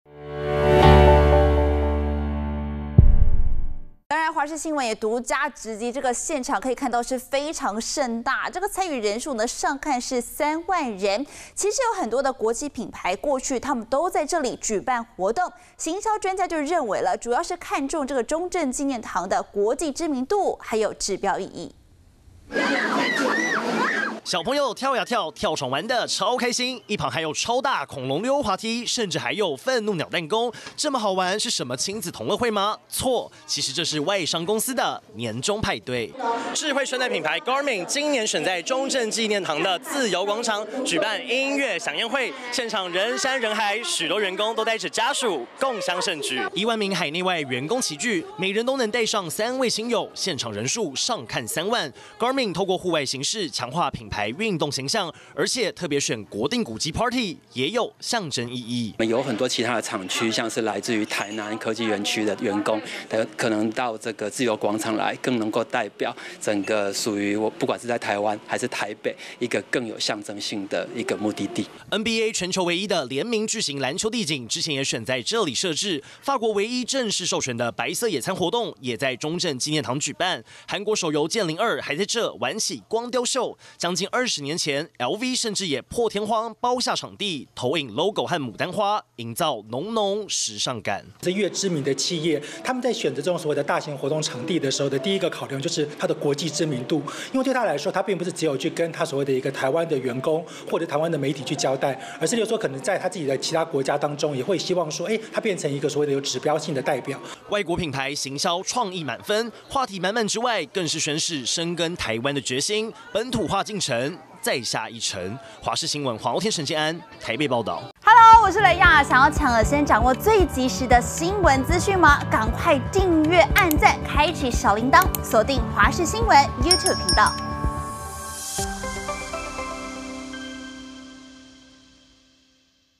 獨家直擊智慧錶廠歲末趴 戶外音樂饗宴近3萬人｜華視新聞 20240120
歲末年終，台灣科技廠陸續舉辦大型活動，絕大多數都在南港展覽館室內進行，但來自美國的智慧穿戴品牌今(20)日的年終派對，不只辦在戶外，地點還選中正紀念堂的自由廣場。華視新聞獨家直擊現場，場面相當盛大，參與人數上看3萬！其實NBA、LV等國際品牌，過去都曾在這舉辦活動；行銷專家認為，是看重中正紀念堂的國際知名度跟指標意義。